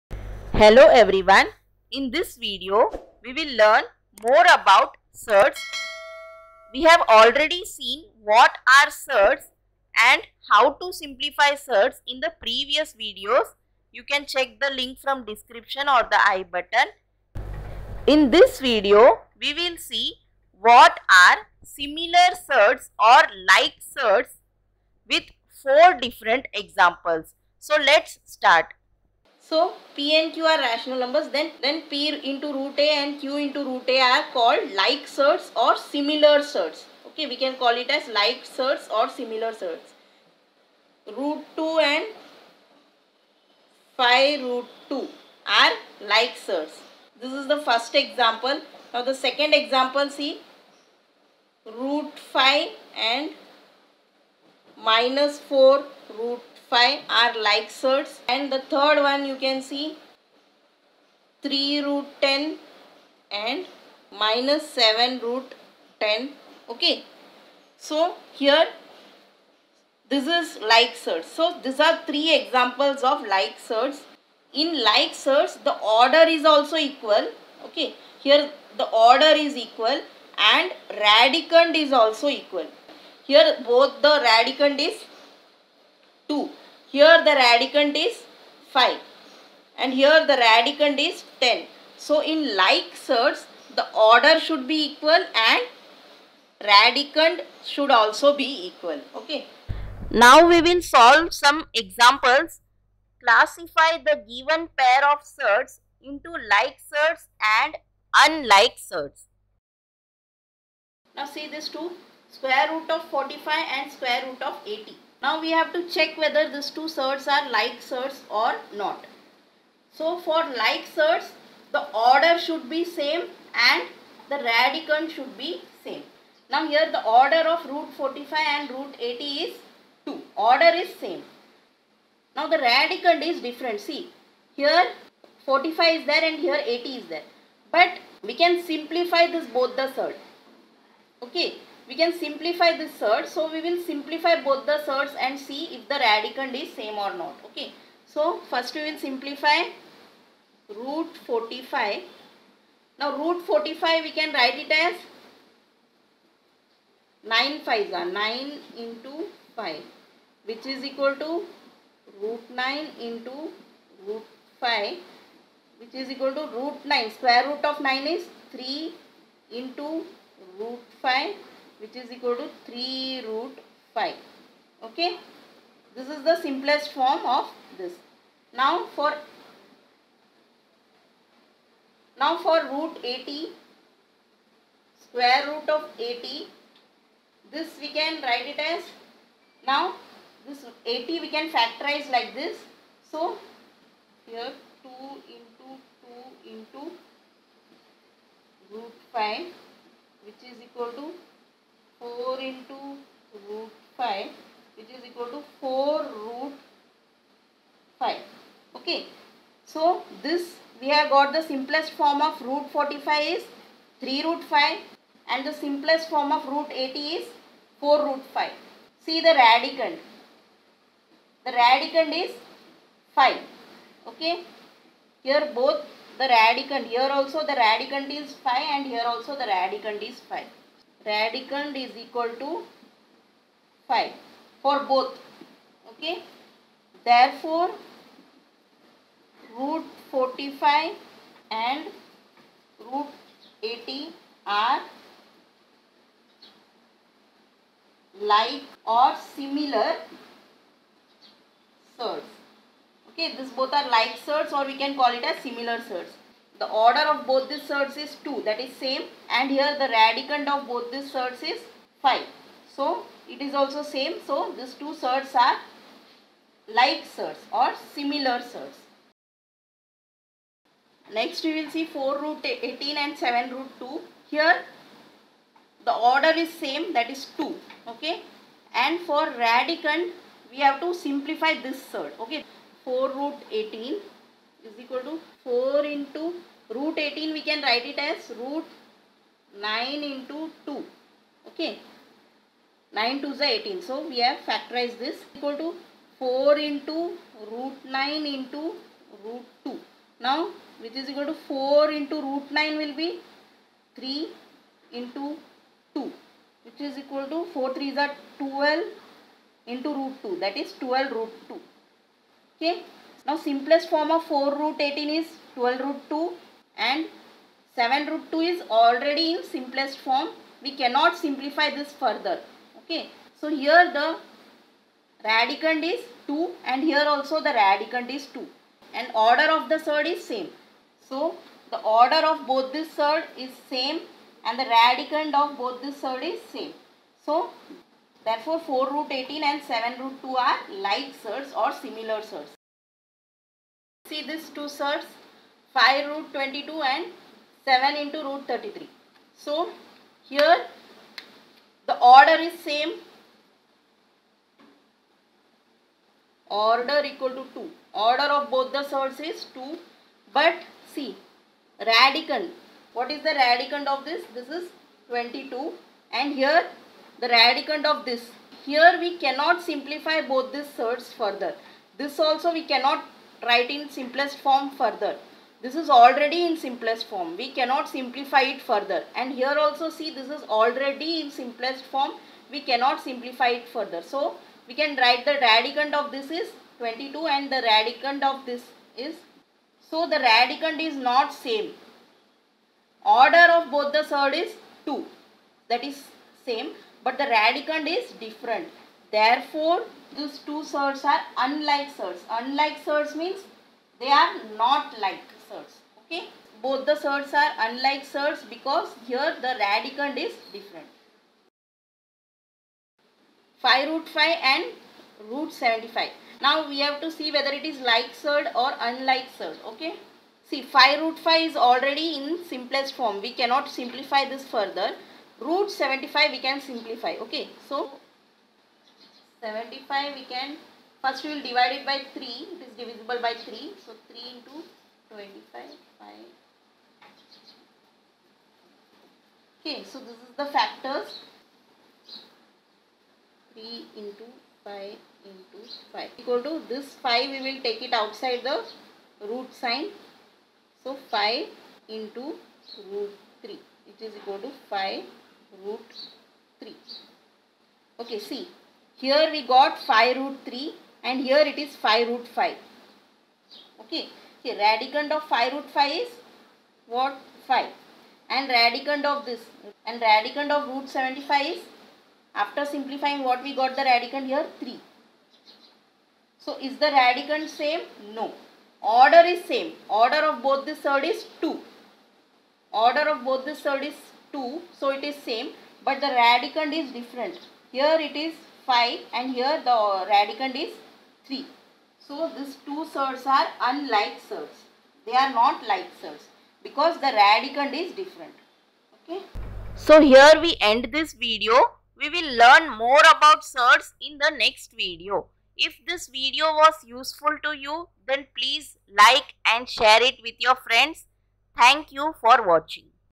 Like Surds or Similar Surds
This video is about Like Surds or Similar Surds.
Classify the given pair of Surds into like surds and unlike surds.